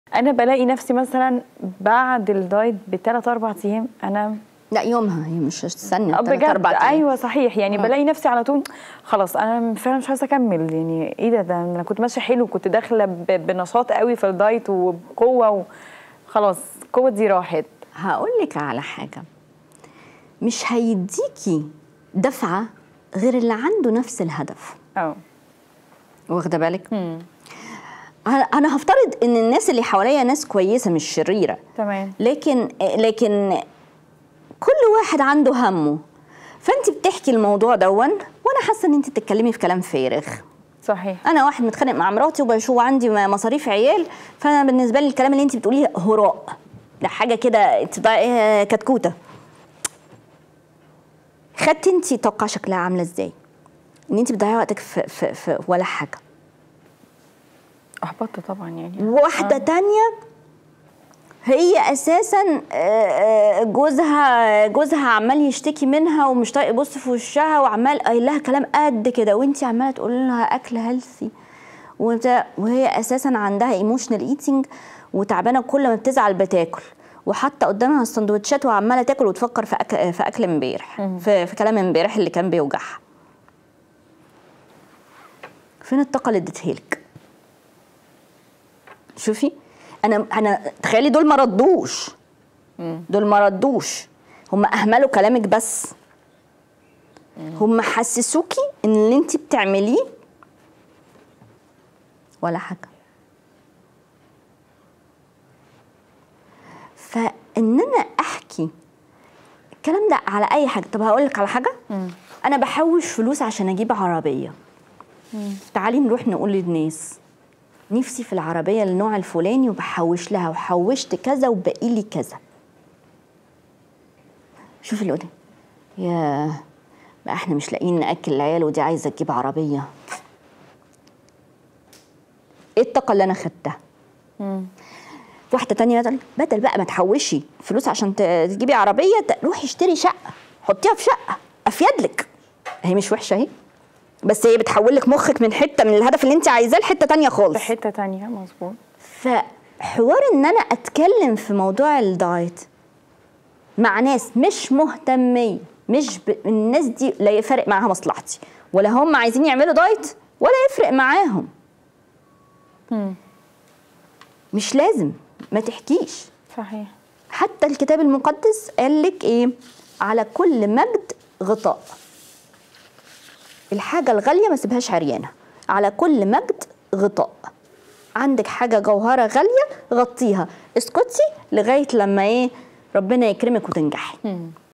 سبب الإحباط المفاجئ في الدايت؟! | بألف هنا
مع د. أميرة سمير "دكتوراه تغذية علاجية موثقة من جامعة كامبريدج - بريطانيا" | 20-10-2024